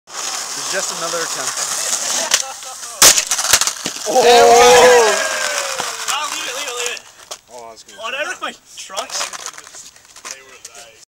Board Snap on Drop
JJ snaping his board after completing a 4-41/2' drop gap